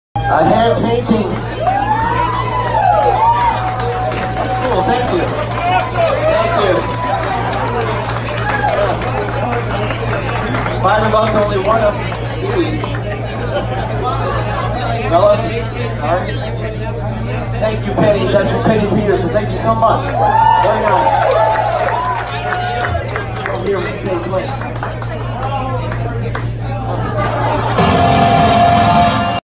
Fatal Malady Painting by Penny Petersen-Freeman
Video by: Trevor Freeman

Painting Presented by Penny to Clinton Rackley / Fatal Malady.